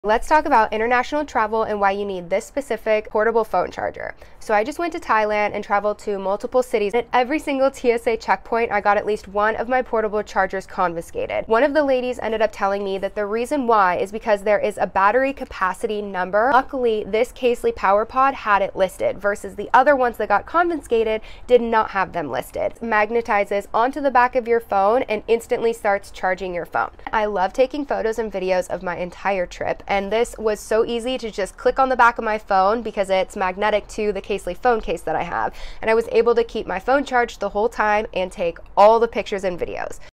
Casely Portable Charger Review | Power Pod MagSafe Portable Charger
Power Pod Specs:
Battery Capacity: 5000mAh
Wireless Output: 15W Max
Charging mode input: USB-C to USB cable (included)
Dimensions: 3" x 2" x 1/4"
Weight: 3.2 oz

Power Pod Compatibility:
Built-in Magnet: Compatible with iPhone 12 & later (with MagSafe® compatible case). Power Pods can be attached to Non-MagSafe® devices & cases using the Adhesive Magnetic Ring (included)
Wireless Charging: Compatible with Qi Wireless Charging Phones, such as iPhone 8 & later, Samsung, Google, Airpods 2, AirPods Pro, AirPods 3
Corded Charging: Compatible with all devices. Requires USB-C charging cable (not included)